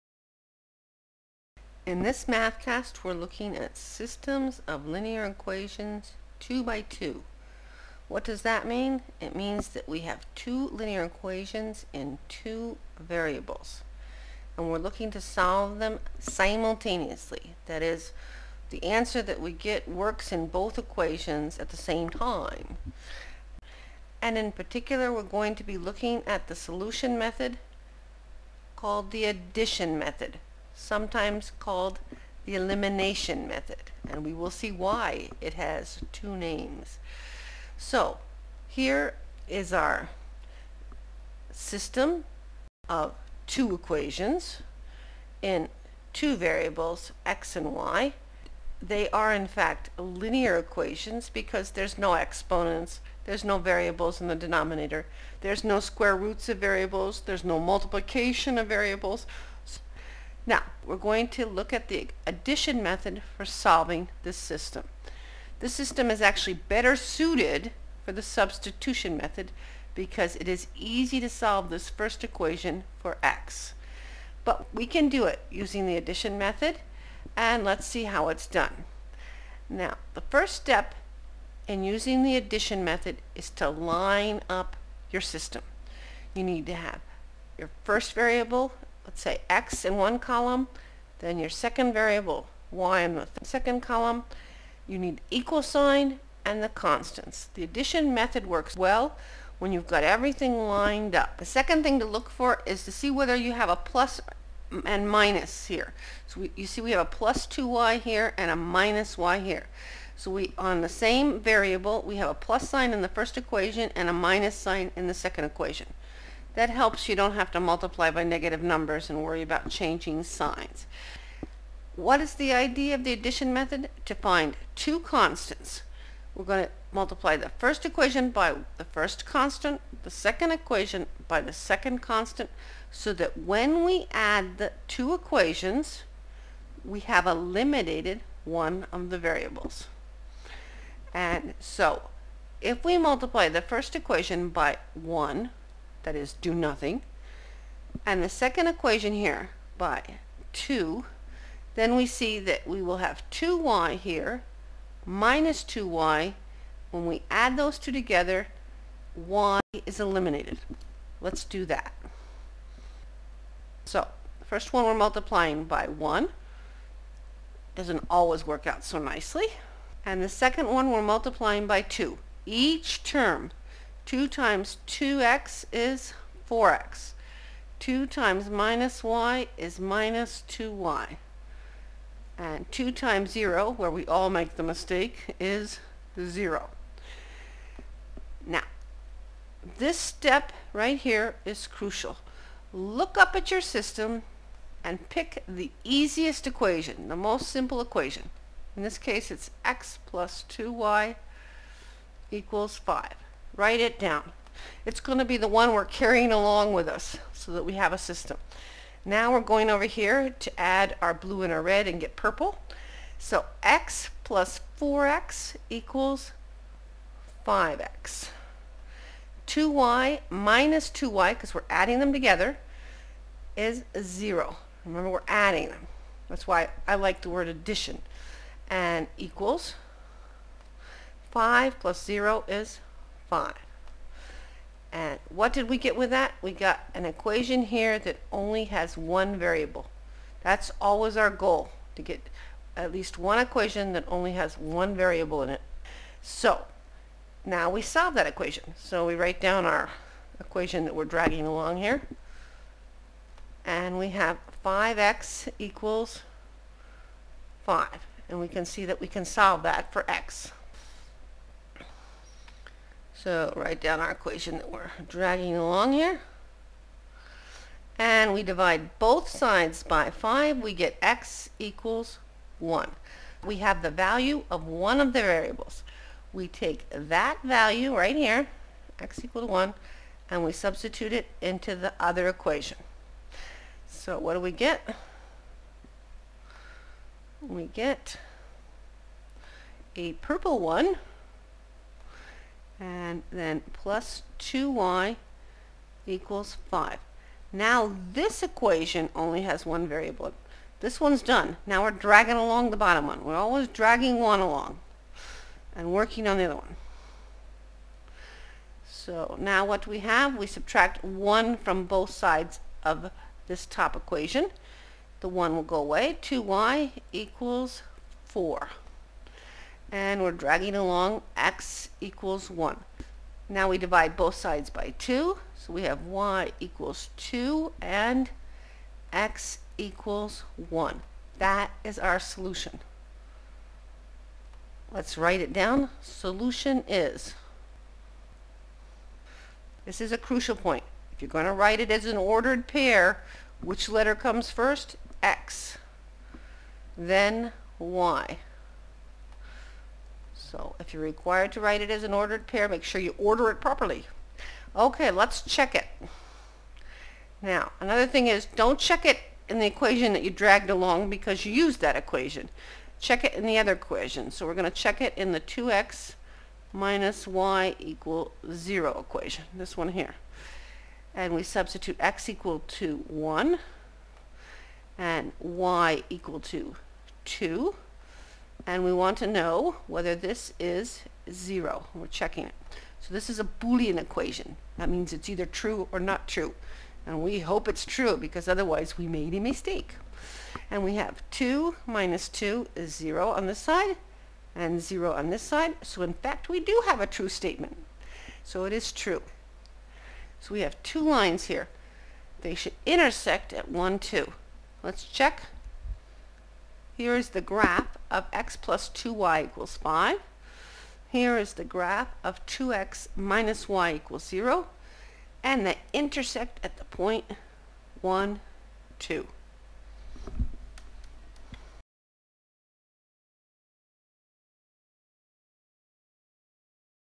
Addition Method ex1: 2x2 Linear System
Step-by-Step: Example 1 - Learn to use the addition method to solve a system of 2 equations in 2 variables (unknowns).